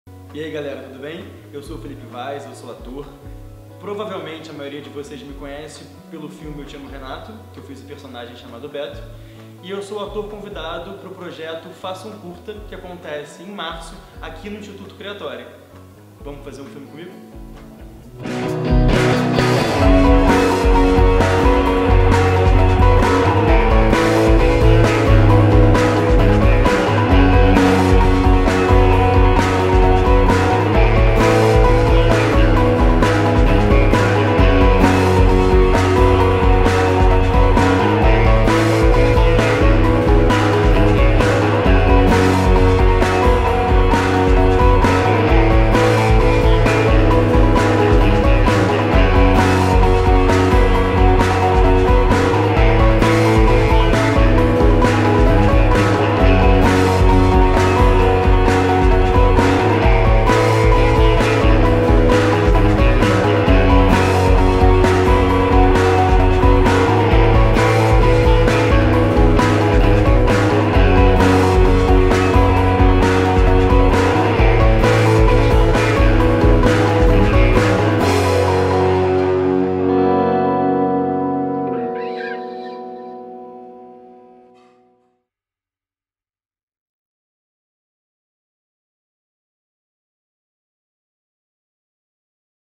Faça Um Curta (08a ed) convida o ator Felippe Vaz (Eu te amo Renato, 2013)
Vamos fazer um filme com o Felippe? O ator Felippe Vaz, o Beto do filme Eu te amo Renato, é o ator convidado para integrar o elenco do curta metragem resultado da 08a edição do projeto Faça Um Curta. Curtiu? Você também pode fazer parte deste filme.